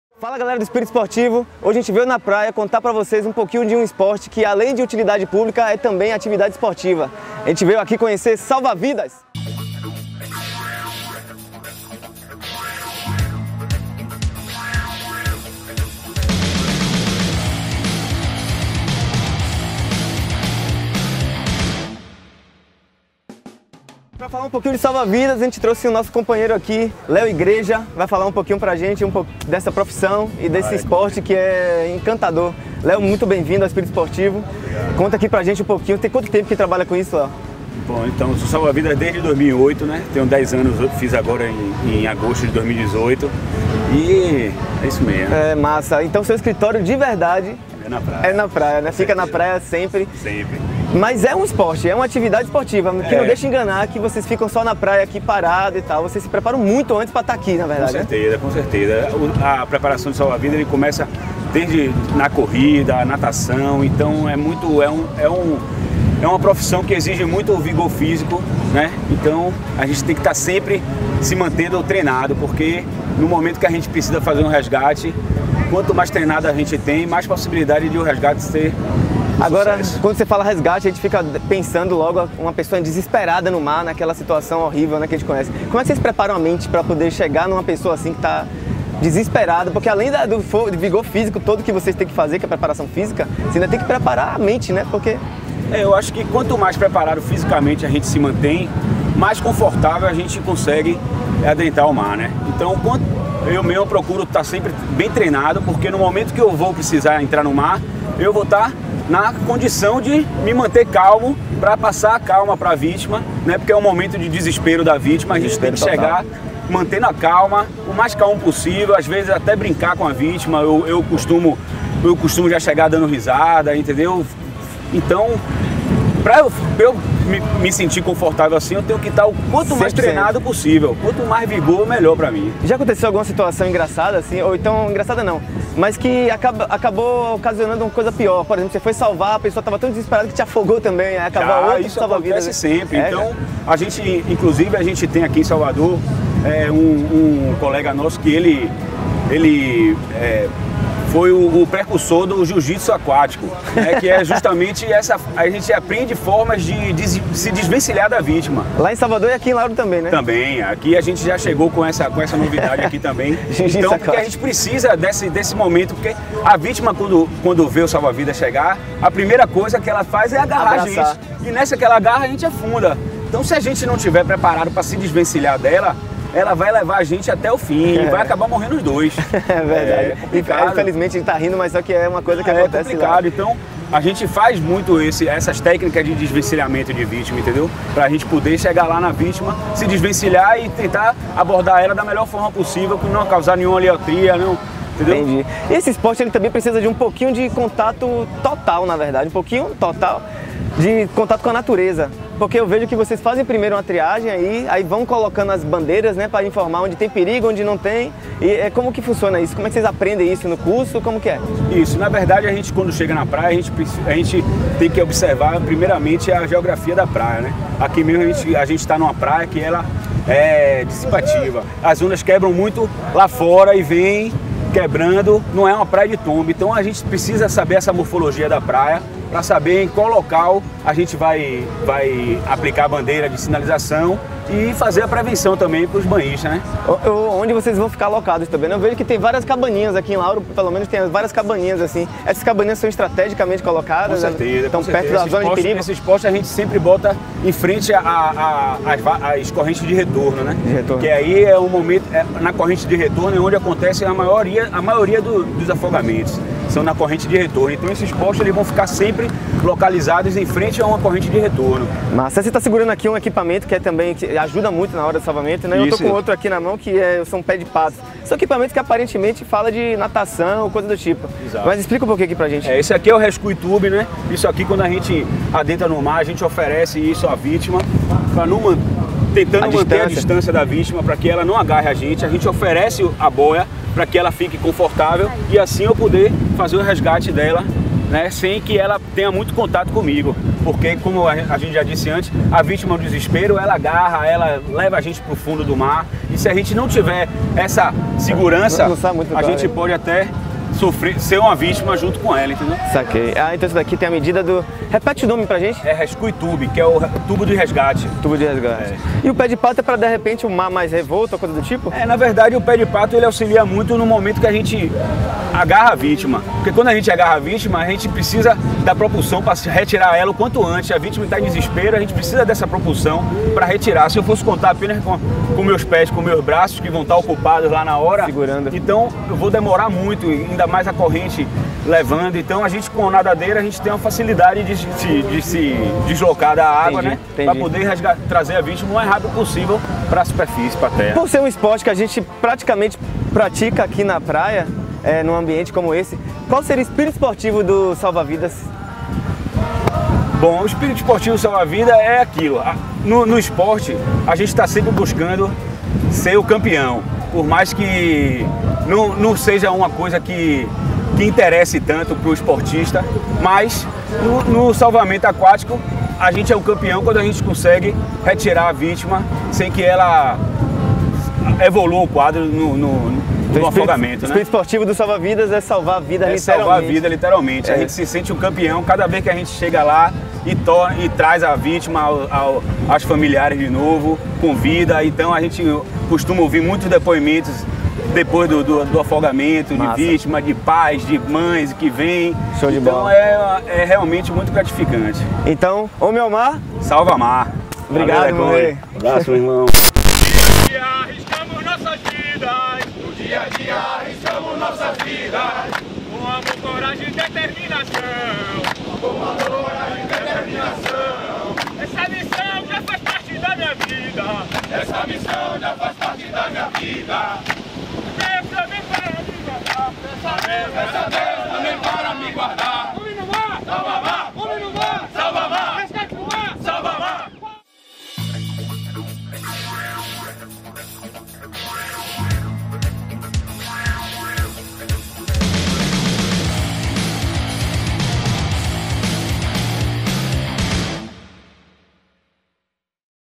Profissão esportiva e heroíca!
SALVA VIDAS
também tem um espírito próprio e é sim...Um esporte!
A segunda temporada está ainda mais radical e com muita novidade para aproximar você de uma vida mais produtiva e saudável, conhecendo o "espirito" de cada esporte que passa por aqui.
Homem ao mar ... SALVAMAR!